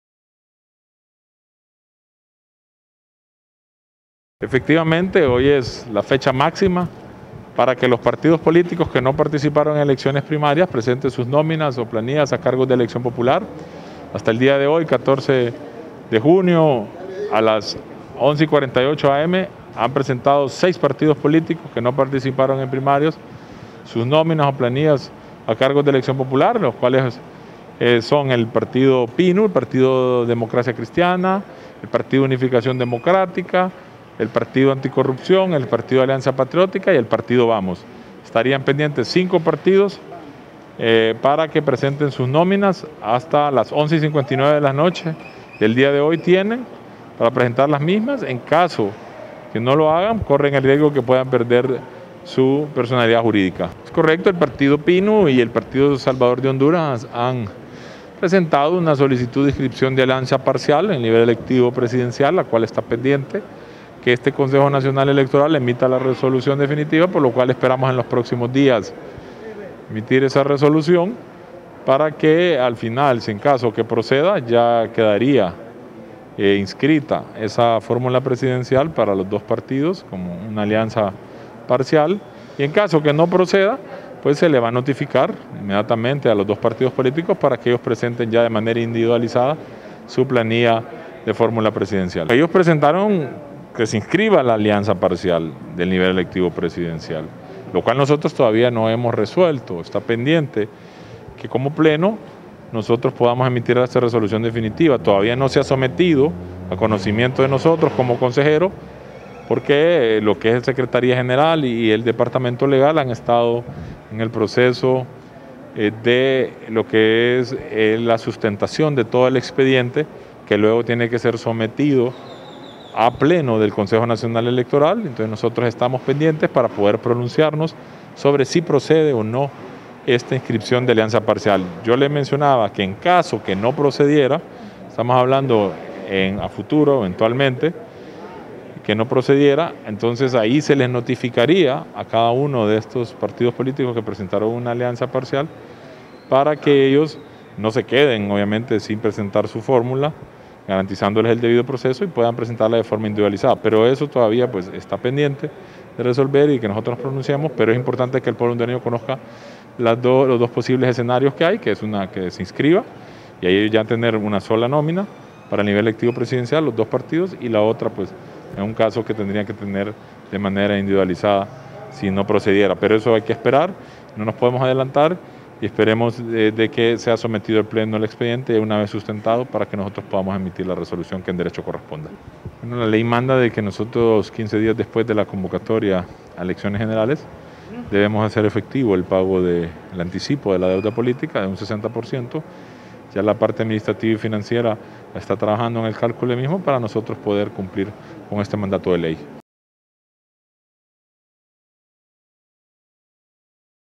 CNE puntualiza que hoy finaliza el plazo para que partidos políticos presenten planillas
El consejero del Consejo Nacional Electoral (CNE), Kelvin Aguirre, puntualizó este lunes que hoy finaliza el plazo para que los partidos políticos presenten las planillas de los comicios generales del próximo 28 de noviembre.